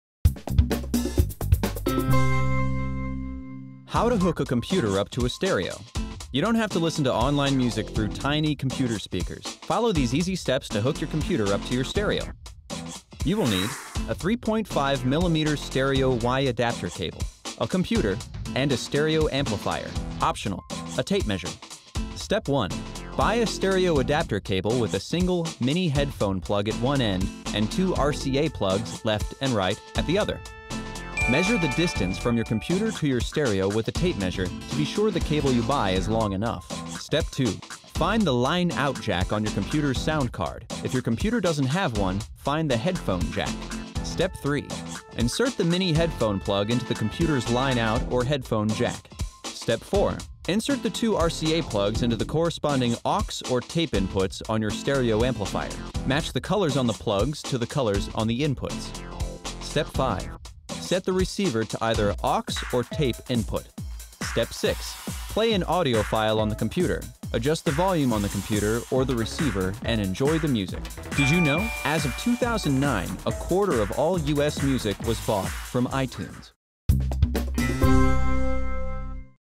How to Hook a Computer Up to a Stereo
You don't have to listen to online music through tiny computer speakers. Follow these easy steps to hook your computer up to your stereo.

Step 1: Buy the right cable
Buy a stereo adapter cable with a single, mini headphone plug at one end and two RCA plugs, left and right, at the other.

Tip
Measure the distance from your computer to your stereo with a tape measure to be sure the cable you buy is long enough.

Step 2: Find the Line Out jack
Find the Line Out jack on your computer's sound card. If your computer doesn't have one, find the headphone jack.

Step 3: Plug in the cable
Insert the mini headphone plug into the computer's line out or headphone jack.

Step 4: Attach to the stereo
Insert the two RCA plugs into the corresponding AUX or TAPE inputs on your stereo amplifier. Match the colors on the plugs to the colors on the inputs.

Step 5: Set up the receiver
Set the receiver to either AUX or TAPE input.

Step 6: Play
Play an audio file on the computer. Adjust the volume on the computer or the receiver and enjoy your music.

As of 2009, a quarter of all U.S. music was bought from iTunes.